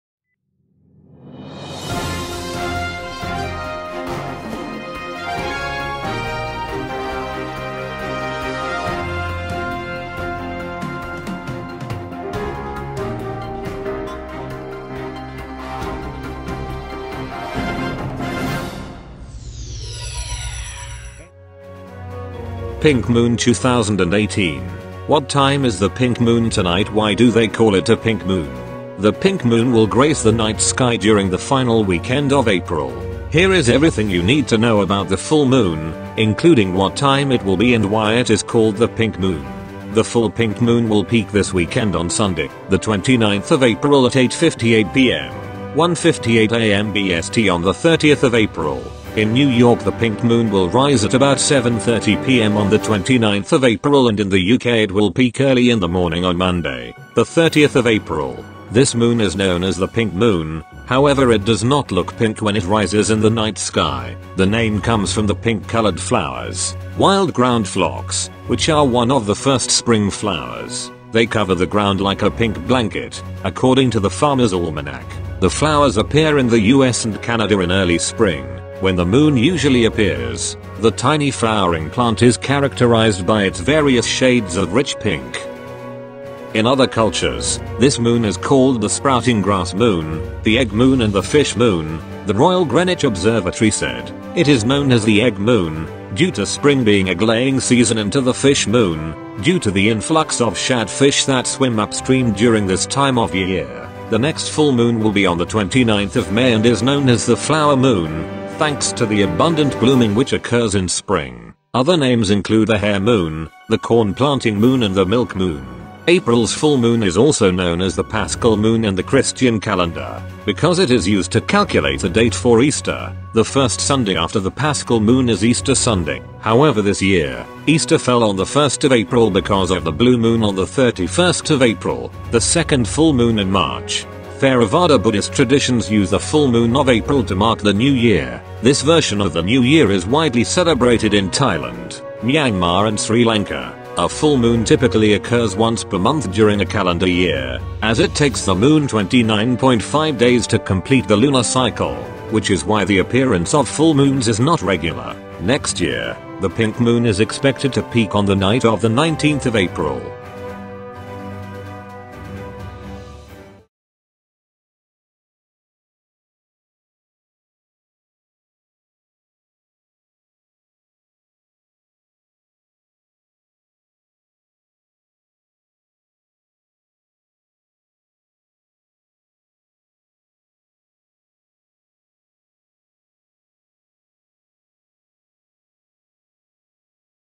Pink Moon 2018 What time is the pink moon tonight Why do they call it a pink moon
The PINK Moon will grace the night sky during the final weekend of April. Here is everything you need to know about the full moon, including what time it will be and why it is called the Pink Moon.